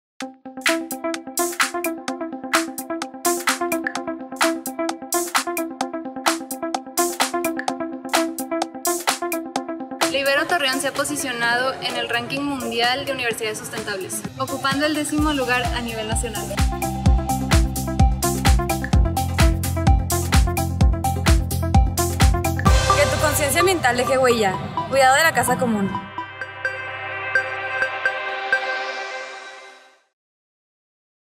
Que tu conciencia ambiental deje huella. Ranking mundial de universidades sustentables.
Campus sustentable.
¿Sabías que...?
Nuestra Universidad se ha posicionado en el ranking mundial de universidades sustentables, ocupando el décimo lugar a nivel nacional.
En la Ibero Torreón cuidamos la Casa Común.